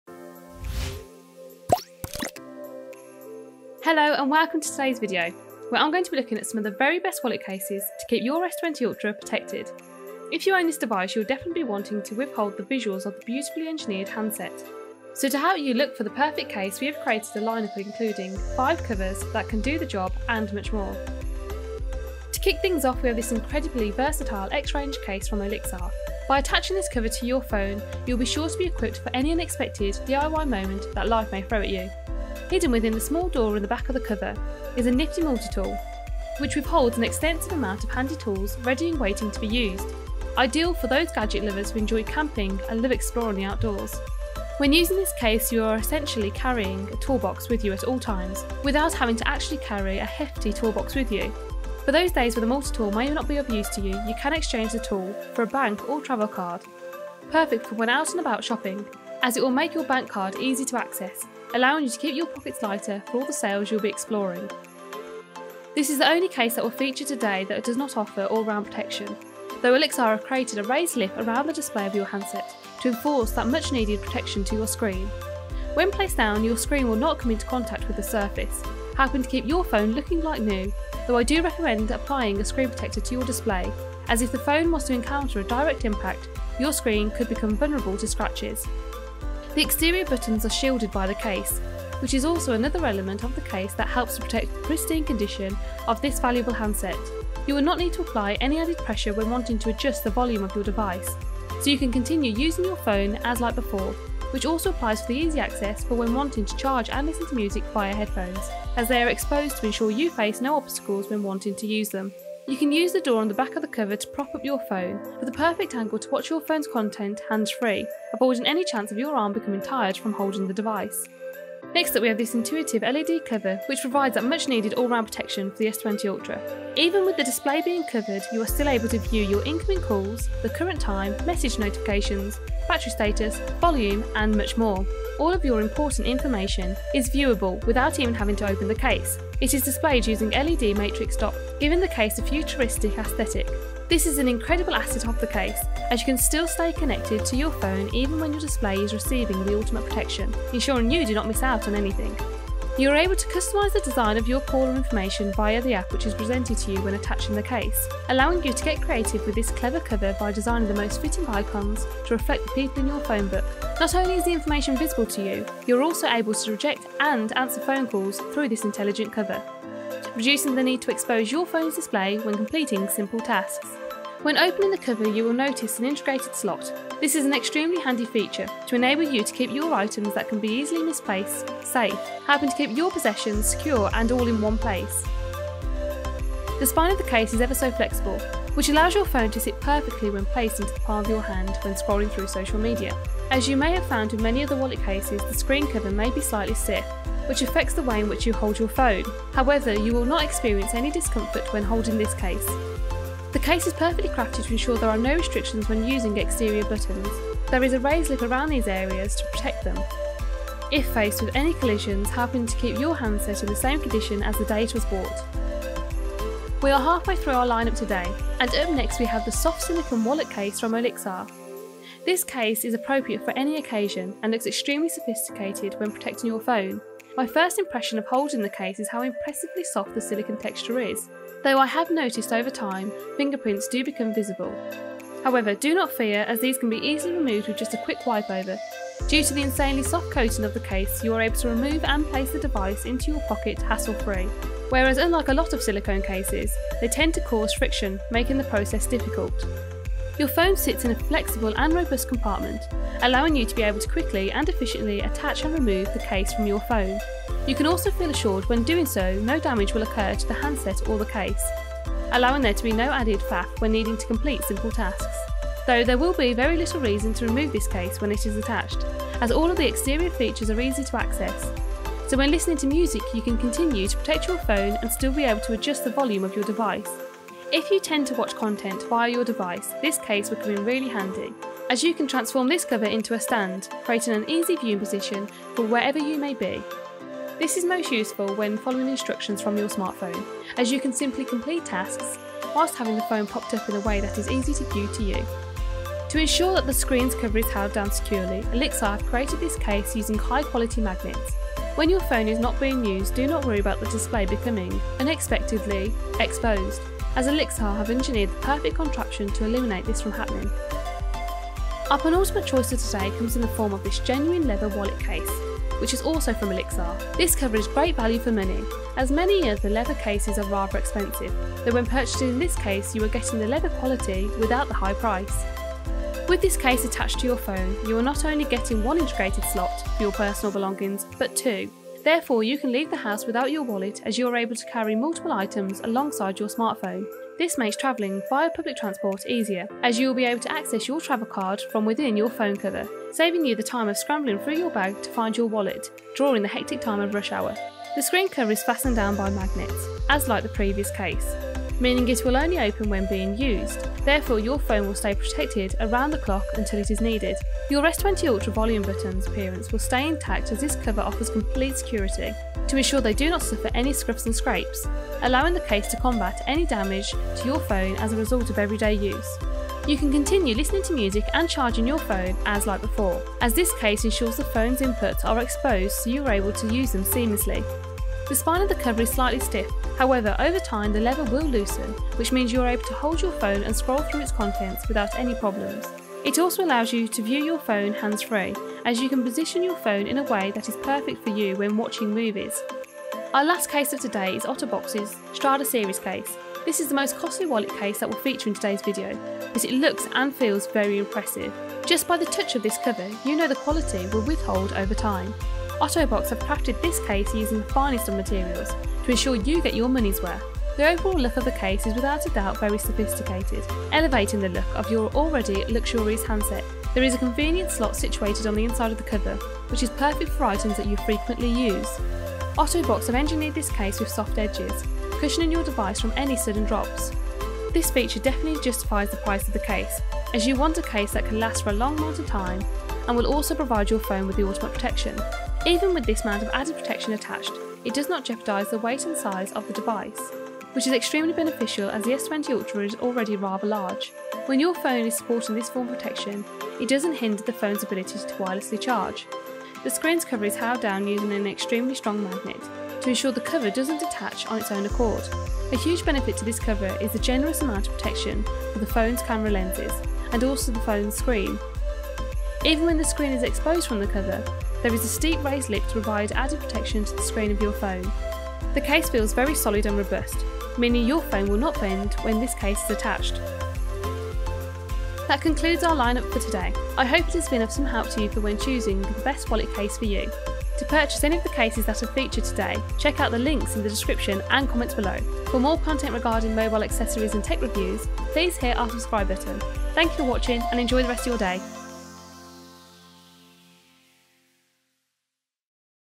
Samsung Galaxy S20 Ultra: Best Wallet Cases
Within this video I'll be taking a look at some of the best wallet case that are available to the Samsung Galaxy S20 Ultra. If you protect your phone using a wallet case you are providing your phone with the ultimate protection as well as keeping your belonging all in one place.

Olixar X-Ranger Samsung Galaxy S20 Ultra Case - Black: (00:24)

Official Samsung Galaxy S20 Ultra LED View Cover Case - Black: (02:01)

Olixar Soft Silicone Samsung Galaxy S20 Ultra Wallet Case - Black: (03:38)

Olixar Slim Genuine Leather Samsung Galaxy S20 Ultra Wallet Case: (05:11)

OtterBox Strada Series Case Samsung Galaxy S20 Ultra - Black: (06:34)

Social Links...